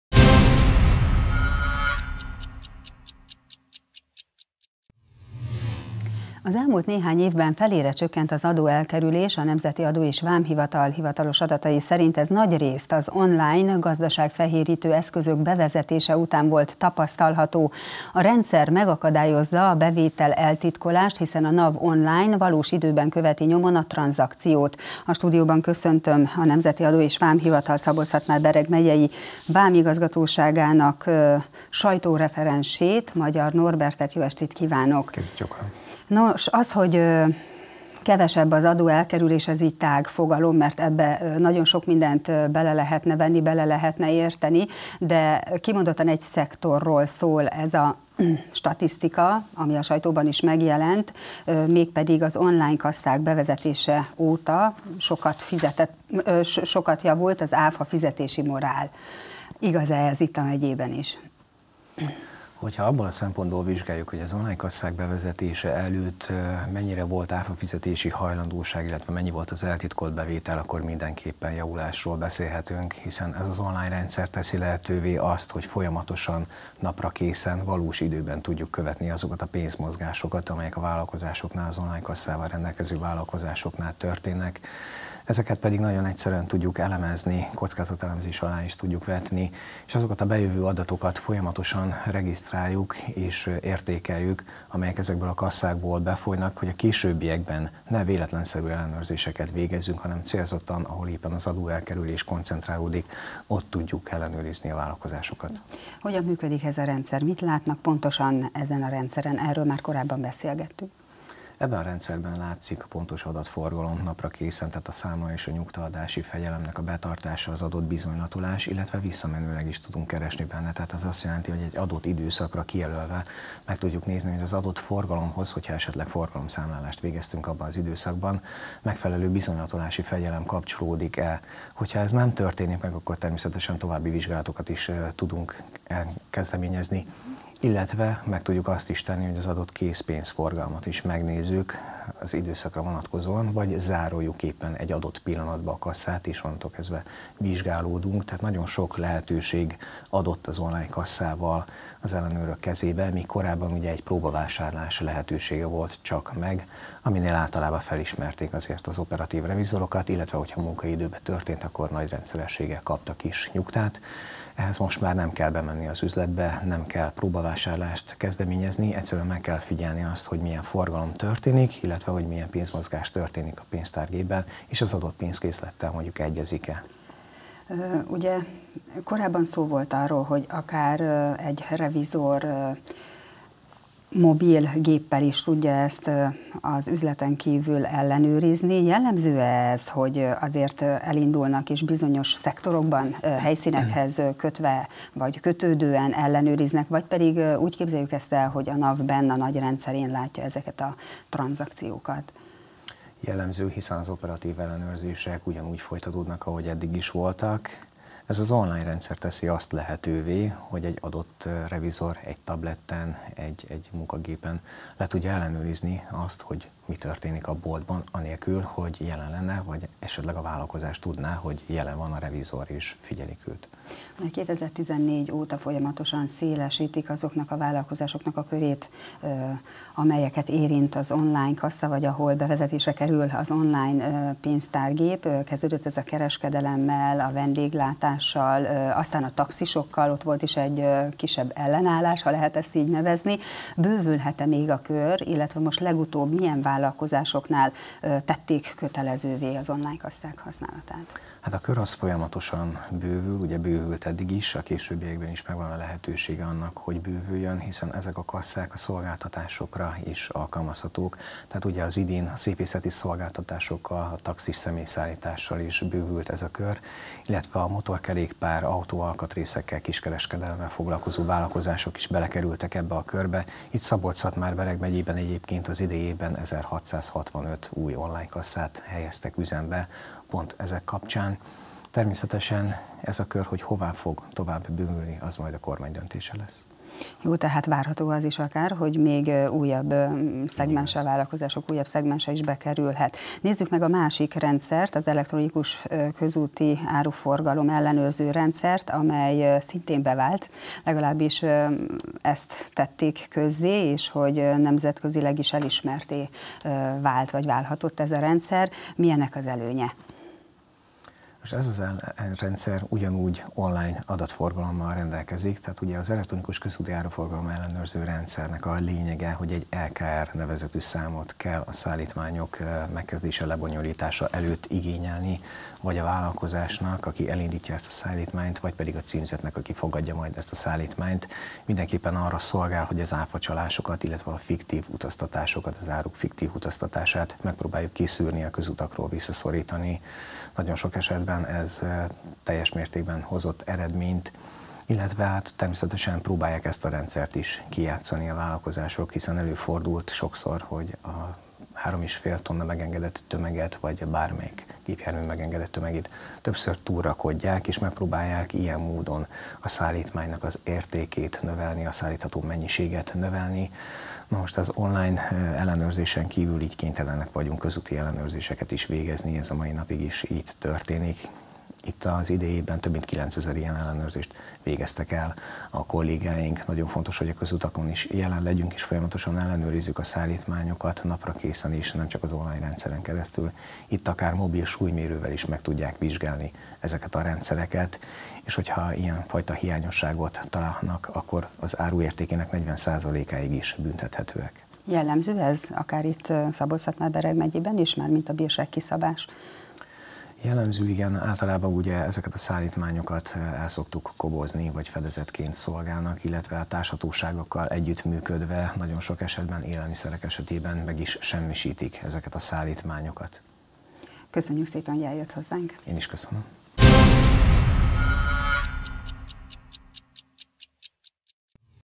Kevesebb az adóelkerülés - Kölcsey Tv
Az elmúlt néhány évben felére csökkent az adóelkerülés- a Nemzeti Adó-és Vámhivatal hivatalos adati szerint. Ez nagyrészt az online gazdaságfehérítő eszközök bevezetése után volt tapasztalható. A rendszer megakadályozza a bevételeltitkolást, hiszen a NAV online, valós időben követi nyomon a tranzakciót.